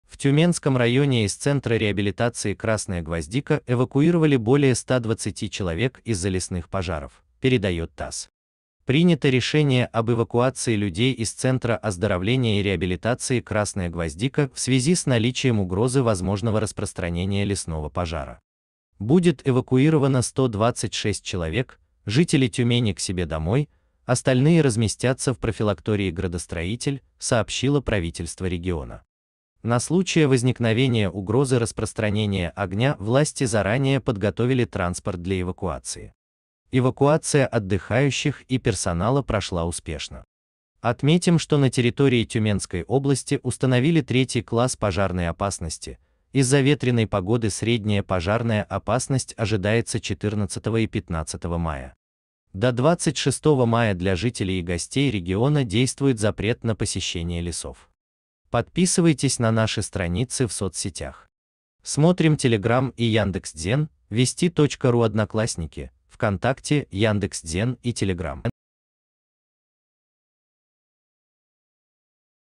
Под Тюменью из центра реабилитации эвакуировали более 120 человек
В Тюменской области эвакуировали работников и отдыхающих центра реабилитации "Красная гвоздика" из-за угрозы возможного распространения лесного пожара. Эвакуация 126 человек прошла успешно.